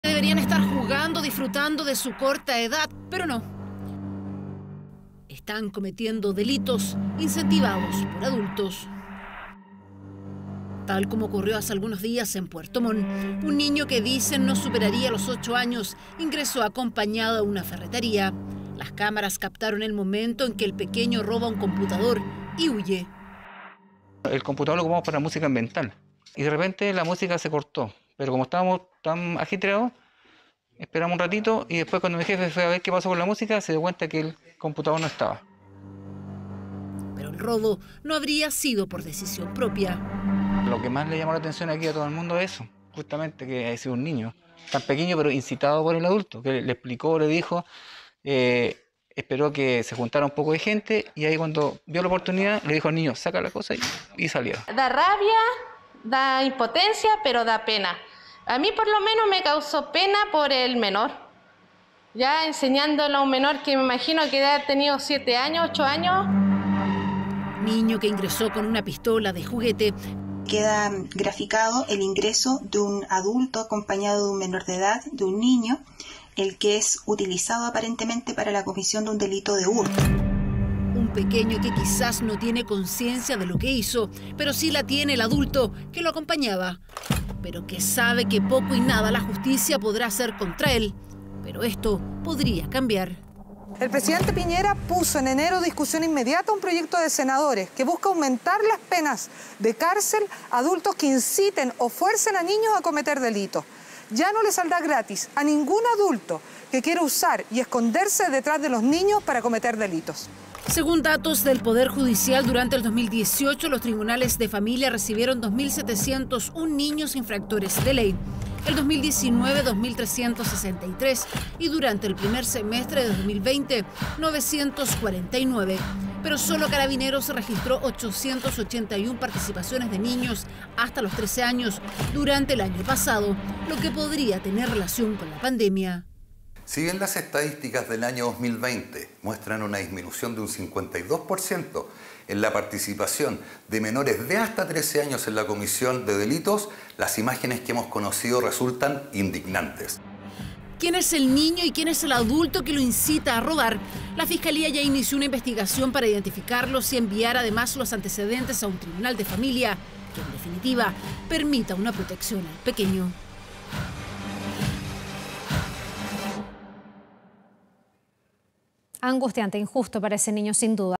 Adulto incita a niño a robar computador en ferretería de Puerto Montt
La cámara de seguridad de una ferretería de Puerto Montt, captó como un niño de no más de ocho años roba un computador de la tienda. Todo incitado por un adulto, que presumen podría ser un familiar.